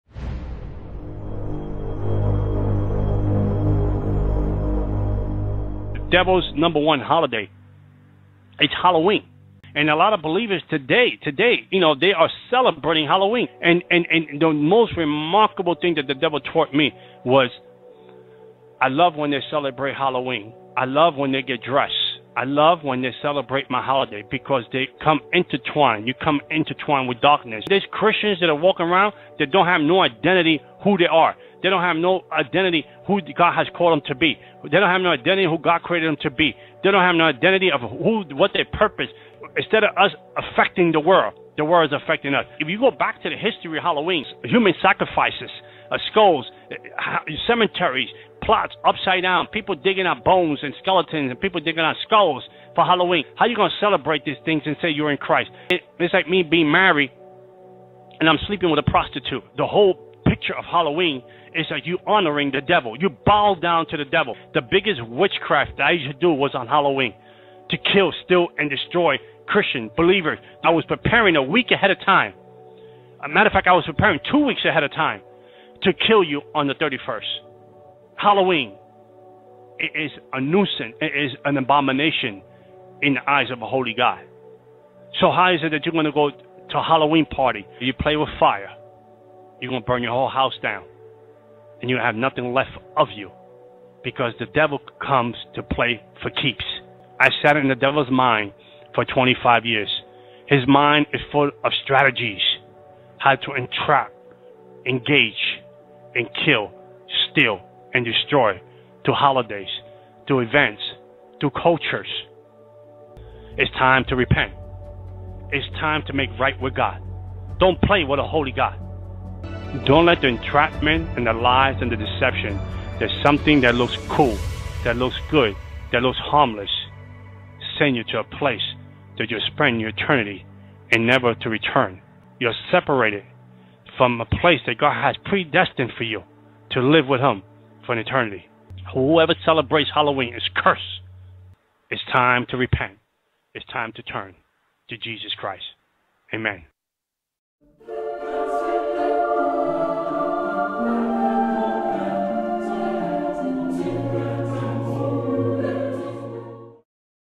Ex-Satanist John Ramirez warnt vor Halloween: "Es ist der Feiertag des Teufels!"
Über 25 Jahre war John Ramirez ein hochrangiger Satanist und sprach öfters mit dem Teufel und seinen Dämonen. Er feierte sogar zu Halloween eine dämonische Hochzeit. Gott hatte aber andere Pläne mit ihm und John Ramirez bekehrte sich später zu Jesus Christus. Er warnt eindringlich davor, Halloween zu feiern. Dieser Tag wurde früher von Kelten namentlich als "Samhain" gefeiert und dabei gab man Menschenopfer. Jeder, der Halloween feiert, verbindet sich mit der dämonischen Welt. Der Teufel und seine Dämonen bekommen hierdurch eine offene Tür, um im Leben der Menschen zu wirken. Selbst Kirchen feiern dieses teuflische Fest. Wir sind in der Endzeit und müssen umkehren zu Gott. Die Menschen rennen der verdorbenen Welt hinterher. Sinnlose TV-Shows, Fußball, Heidi Klums Topmodel Unsinn usw. dominieren das Abendprogramm der Menschen. Hinzu kommen Horrorfilme und anderer perverser  Mist, welcher die Menschen in die Arme des Teufels treibt. Als Christ muss man die Menschen warnen und vor allem nicht selbst an solch schändlichen Dingen teilnehmen. Die Endzeit schreitet voran wie man in Israel mit der Hamas sieht. Die Gefahr eines Dritten Weltkrieges war seit dem Kalten Krieg nicht mehr so groß. Kehre um zu Gott, bevor es zu spät ist. Gott hält die Zügel dennoch in der Hand und wird das Antichrist-System zum Einsturz bringen. Bitte schaut auch Predigten von Karl-Hermann Kauffmann, Olaf Latzel und Jakob Tscharntke.

Mein lieber Freund wir leben in der Endzeit und Jesus Christus wird bald wieder kommen. Lies die Bibel.